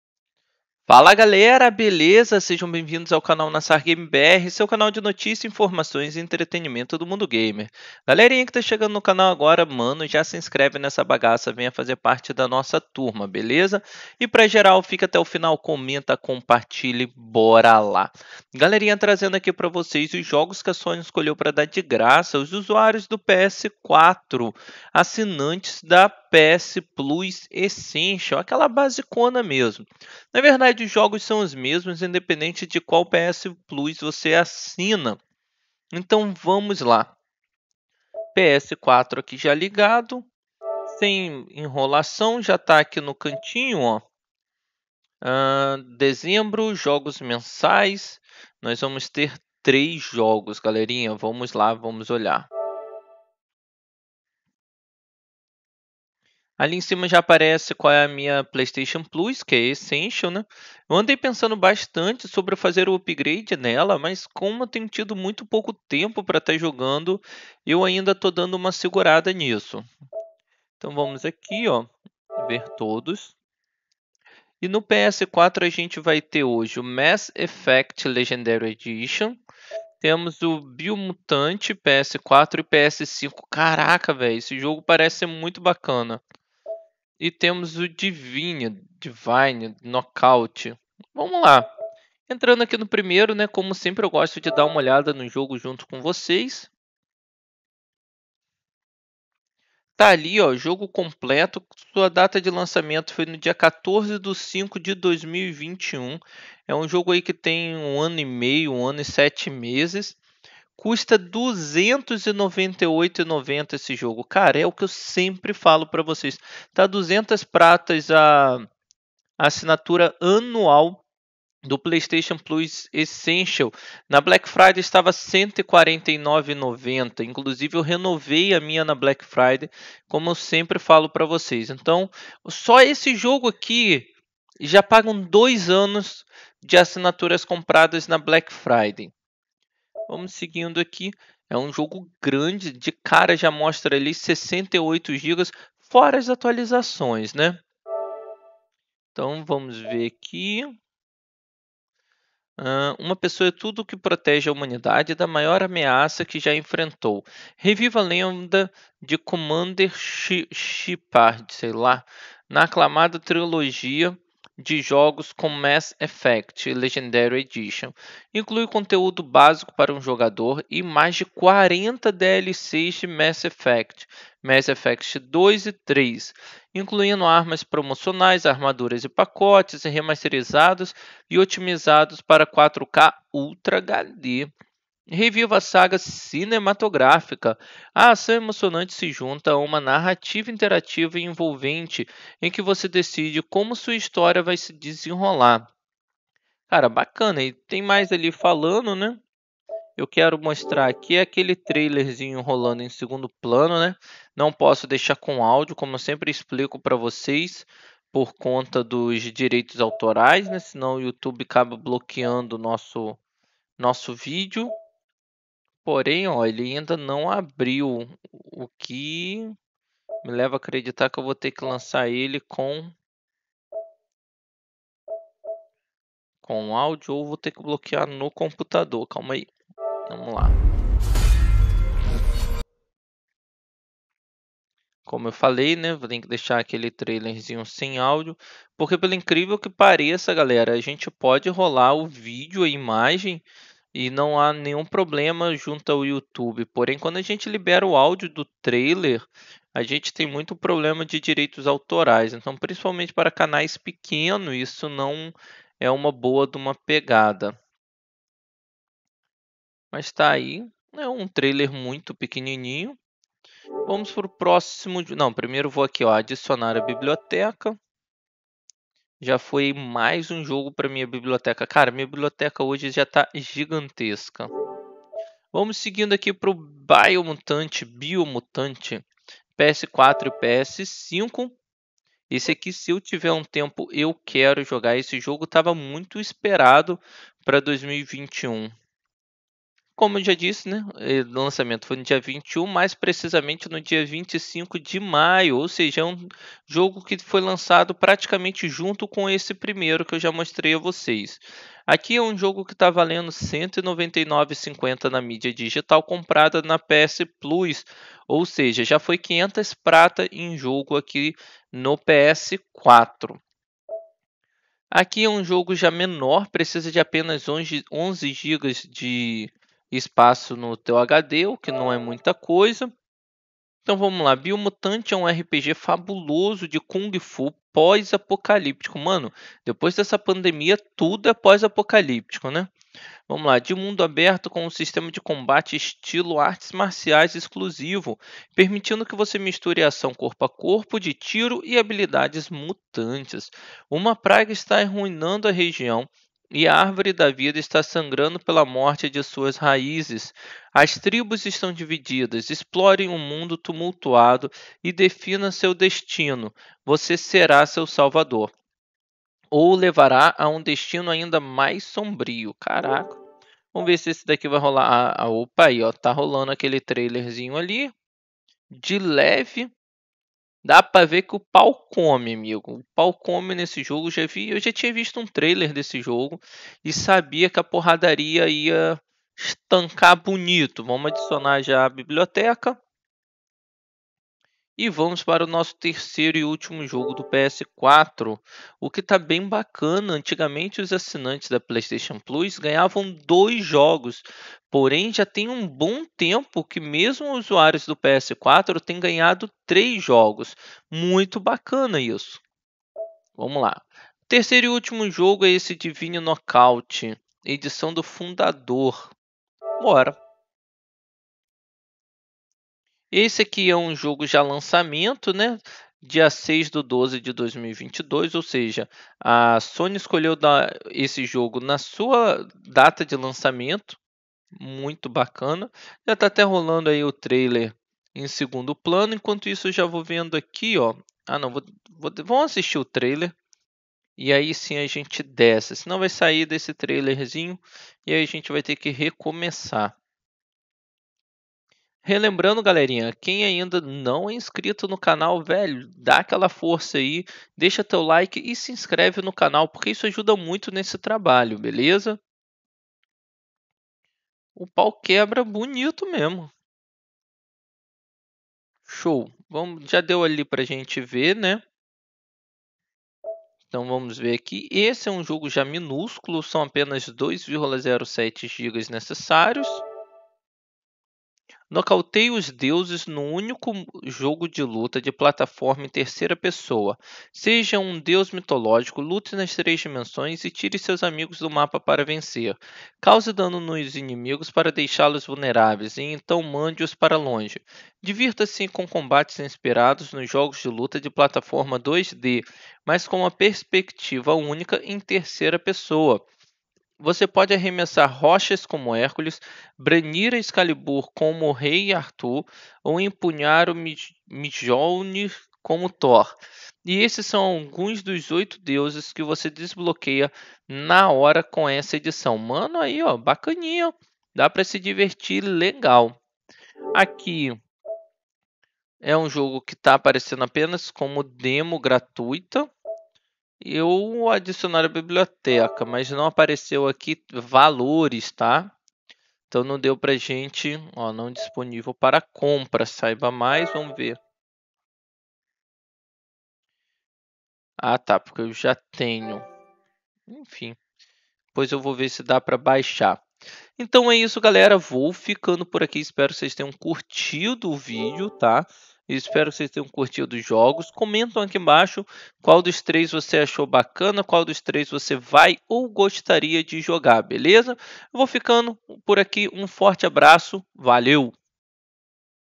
PS4 LIBERA TRÊS JOGOS GRÁTIS EM DEZEMBRO PARA ASSINANTES DA PS PLUS ESSENCIAL
Sejam bem-vindos ao canal Nassar.Gamer.Br! Aproveitem para se inscreverem e dá aquele joinha maroto! Sigam nosso Instagram @Nassar.Gamer.Br para estar sempre sabendo das novidades.
Fala Galera, beleza? A Sony liberou os jogos grátis do mês de dezembro, para os assinantes da PS Plus no PS4, dando simplesmente muita grana em jogos totalmente grátis para seus assinantes.
Como eu sempre digo galera, é praticamente obrigatório ser assinantes da PS Plus!!!
Assista até o final para saber os detalhes.